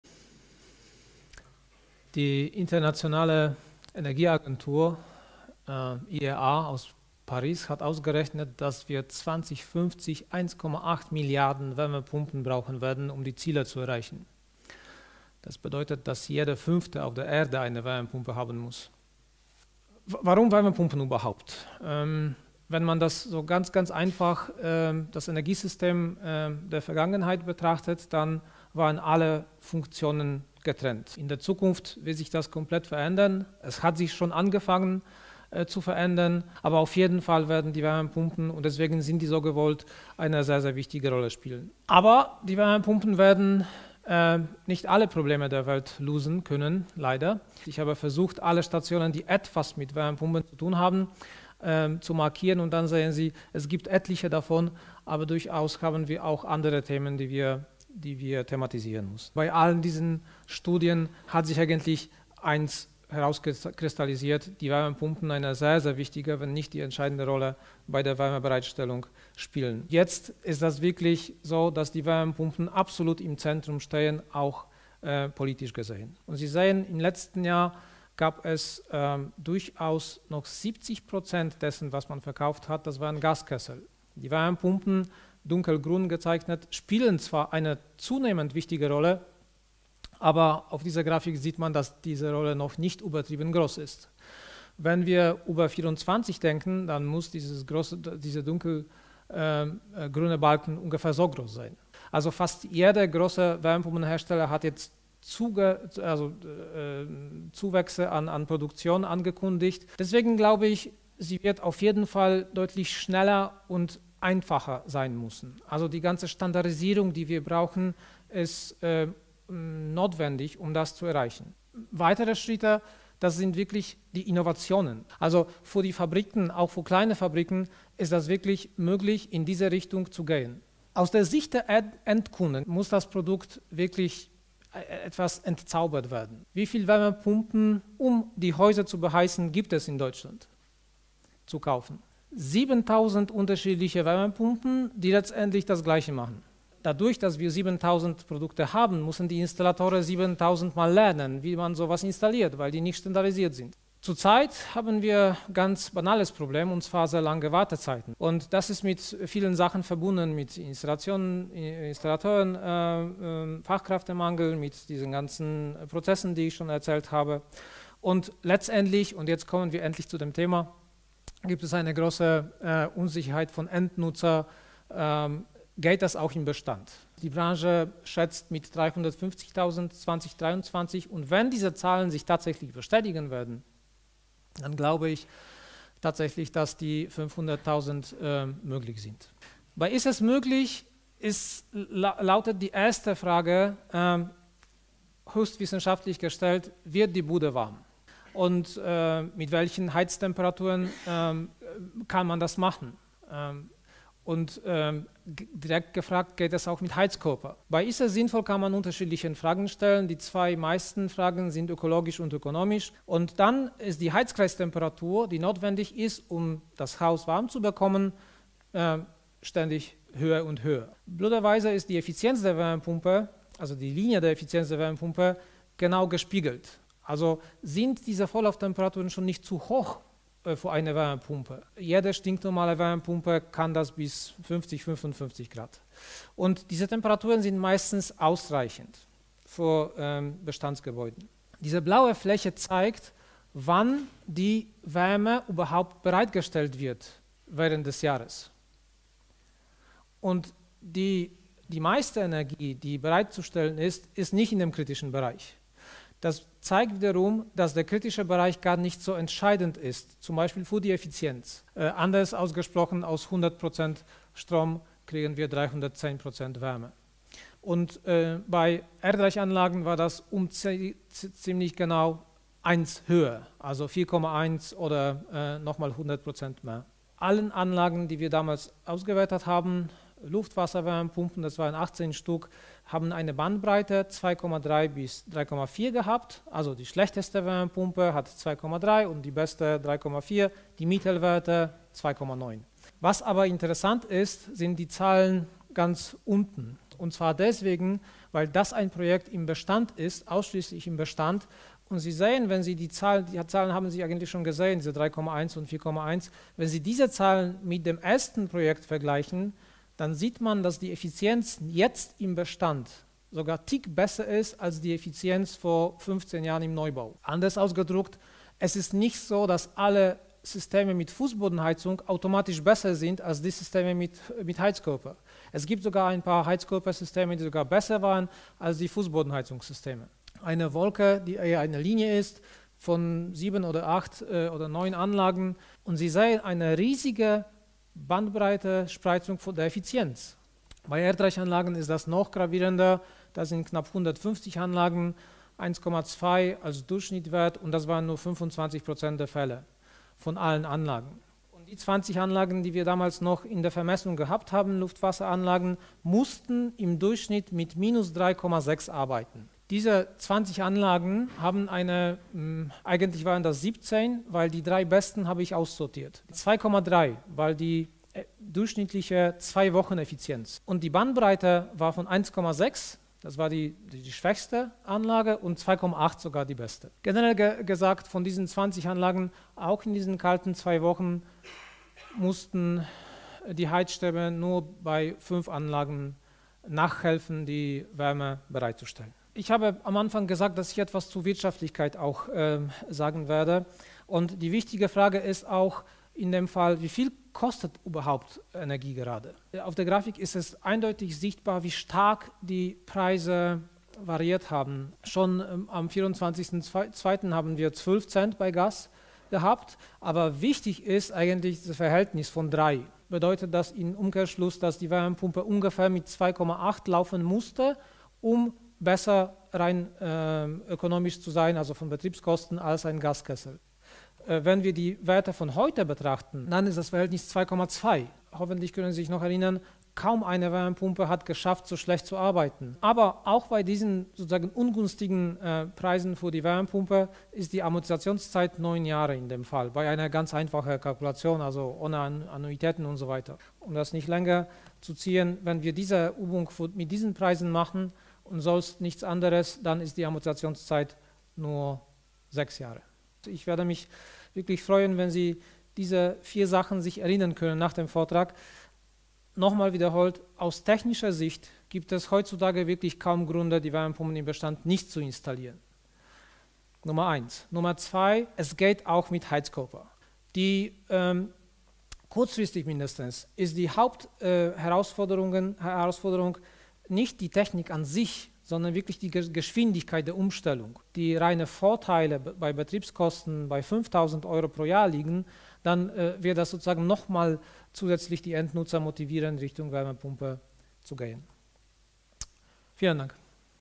Summary: Dr. Marek Miara Wärmepumpen im Bestand - es geht doch!
VDI-Karlsruhe
Die Zusammenfassung wurde aus studentischer Sichtweise (Mechatronik HKA) erstellt.